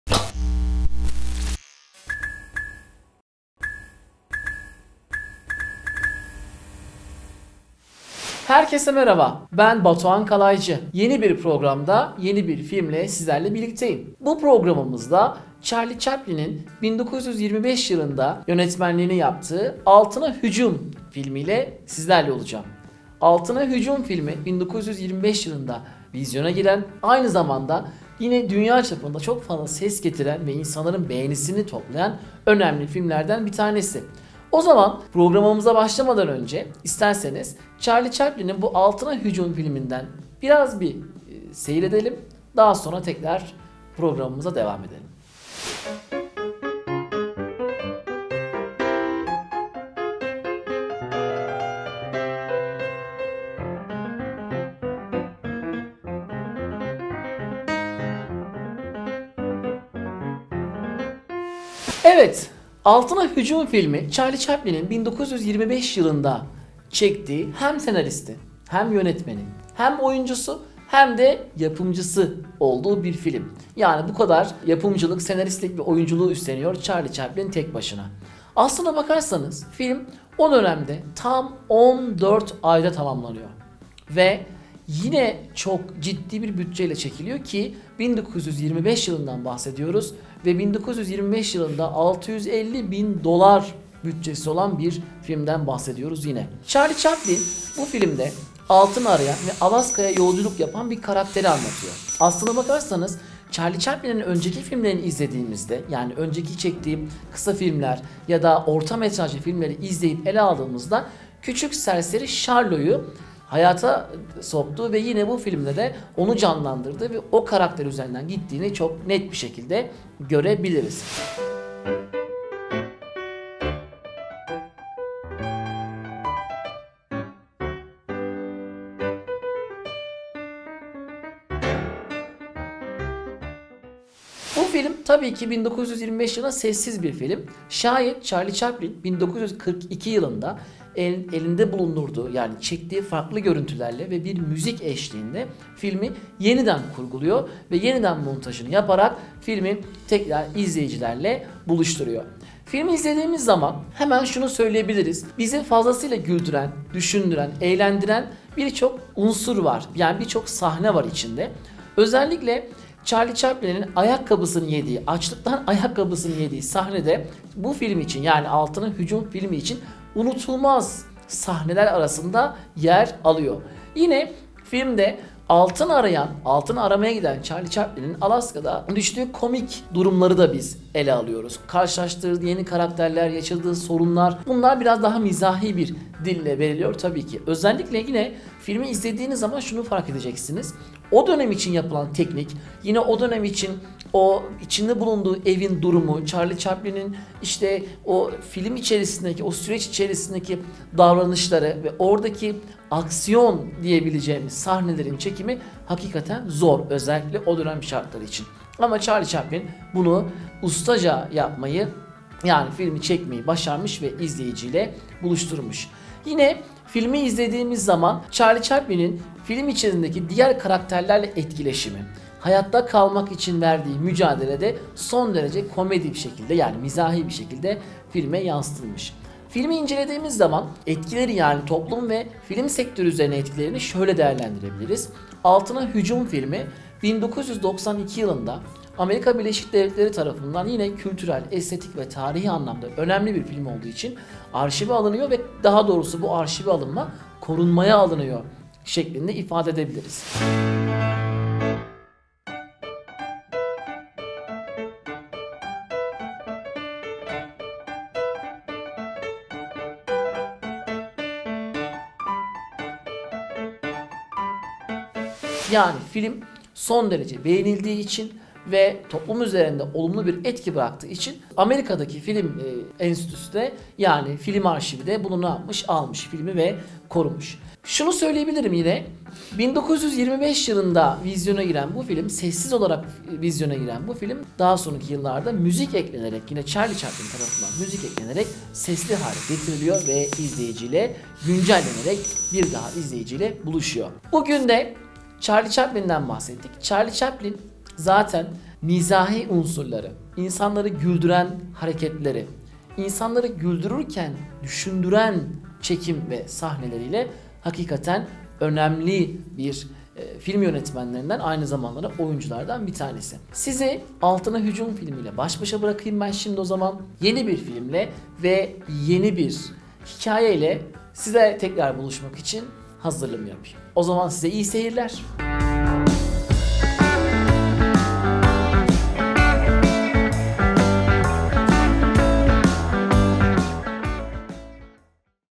14 AYDA TAMAMLANAN FİLM - ALTINA HÜCUM / THE GOLD RUSH
Altına Hücum 1925 ABD yapımı sessiz komedi filmi. Altın aramak için yola koyulan bir adamın hikayesini anlatan filmin özgün adı The Gold Rush olarak geçiyor. Altın aramak için Klondike altın madenleri bölgesine giden ‘The Tramp’, burada çeşitli insanlık dramıyla ve zorluklarla karşılaşır. Kötü hava koşulları nedeniyle bir kulübede başka bir altın arayıcısı ve kaçak bir mahkumla yolları kesişen Şarlo kısa zamanda trajik olaylara şahit olur.

Filmin en unutulmaz sahnelerinden biri de kendi gibi iki altın arayıcıyla birlikte kar fırtınasının ortasında uçurumun kenarına sürüklenmiş bir kulübede mahsur kaldıkları sahnedir. Ayrıca yemek niyetine "ayakkabısını yediği" sahne de akıllara kazınan ve sinema tarihine geçen sahnelerden biridir.

Yapıt aynı zamanda Charlie Chaplin’in en önemli filmlerinden biridir. Filmin devamını ve içeriğini sinemakurdu Batuhan Kalaycı’dan dinleyelim.